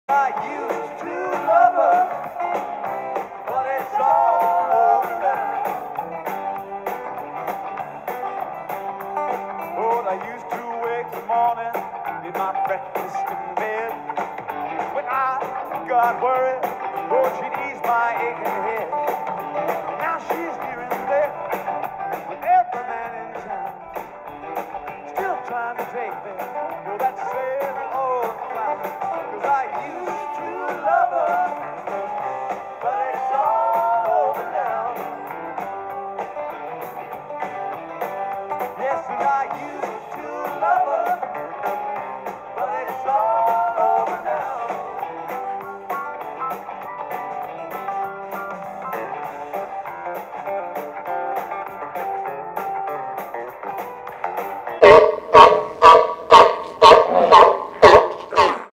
BANDED KETTLEBELL SWINGS (5 X 20 BARN BURNER)
IT'S ALL OVER NOW AFTER THESE BAD BABY'S!

HAMMER GLUTES, LOWER BACK AND BUILD EXPLOSIVE POWER WITH MINIMAL EQUIPMENT

36LB KETTLEBELL WITH MEDIUM RESISTANCE BANDS

52LB KETTLEBELL WITH LIGHT RESISTANCE BANDS